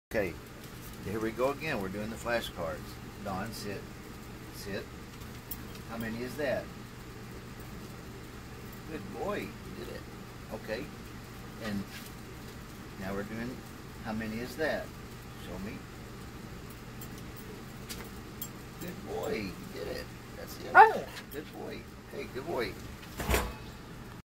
Don Quixote reading numbers🙏🏻❤
Don Quixote is so intelligent, I'm proud to show how intelligent he is I love my dog too. 🙏🏻❤